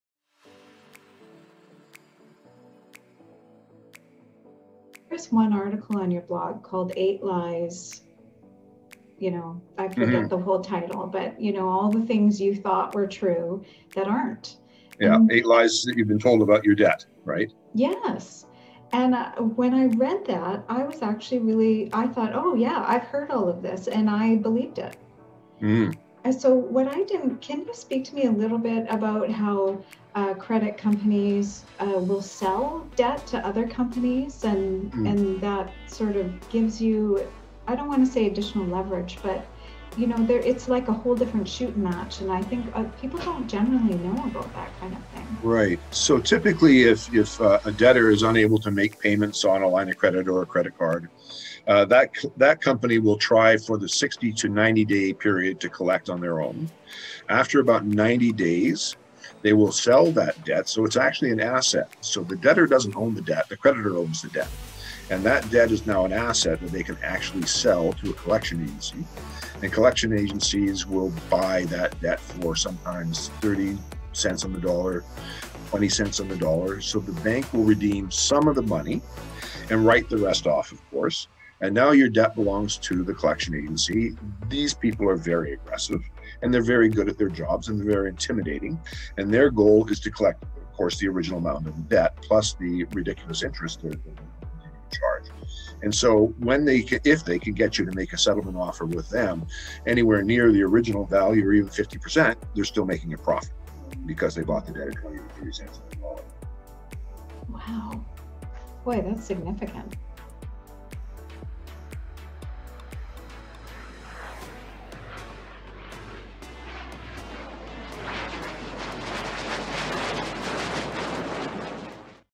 Big Business! Creditors Sell Your Debt?
Debt collectors calling? Do you have any leverage?  Did you know that your unsecured debt (car loans, high-interest loans, and other small debts) can be sold and passed around to different collection agencies? Once a consumer goes into default and starts skipping payments, they become a high-risk investment.  Large firms will typically sell this debt for pennies on the dollar.

How to deal with debt. How to deal with debt collectors.